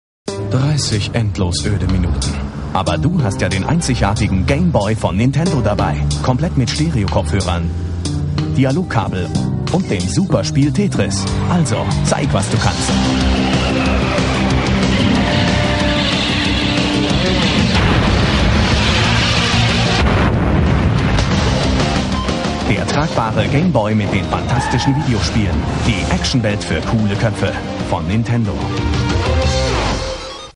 Nintendo Game Boy Werbung (Fixed Video / Audio)
Original:
Archaeen - Nintendo Game Boy Werbung

Thumbnail:
Vintage Original Retro 1990 Nintendo Game Boy Tetris Consumers Color ad

Untertitel auch noch gefixt.